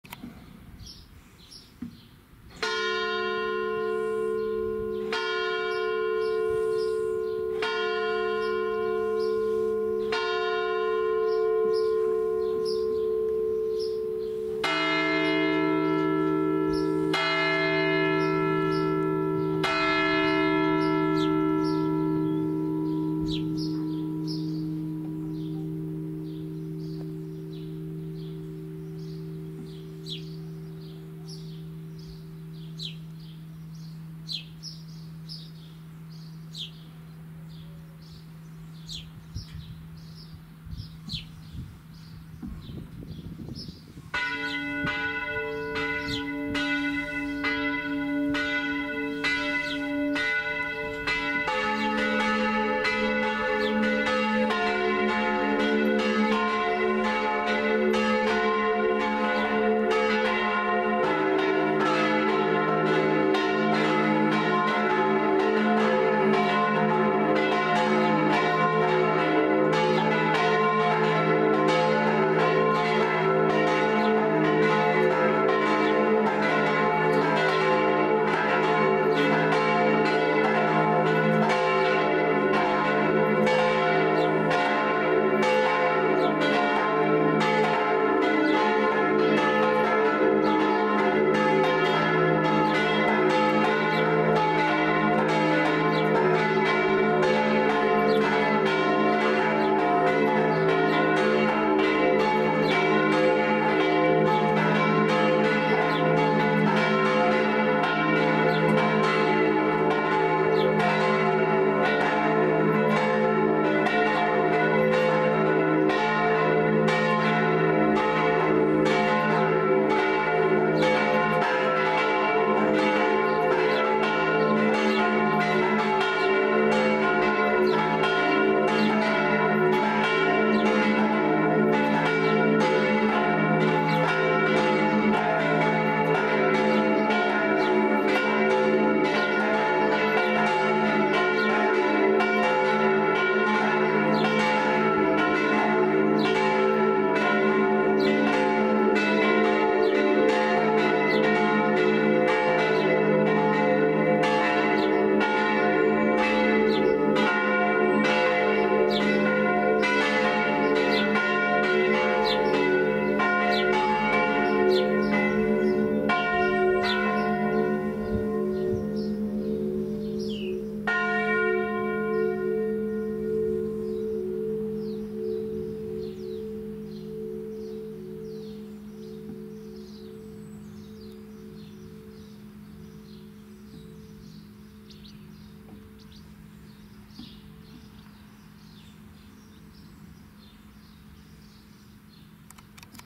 Feldkirchen (SR) - Pfarrkirche St. Laurentius Vollgeläut zur Sterbestunde Christi am Freitag 15:00
Es läuten die vier Glocken der katholischen Pfarrkirche St. Laurentius in Feldkirchen im Landkreis Straubing-Bogen in Niederbayern.

Disposition: e‘ fis‘ a‘ h‘ (verzogen)

Gießer: Gl. 1,3 und 4 von Anton Gugg in Straubing im Jahr 1951. Gl. 2 von Schelchshorn in Regensburg.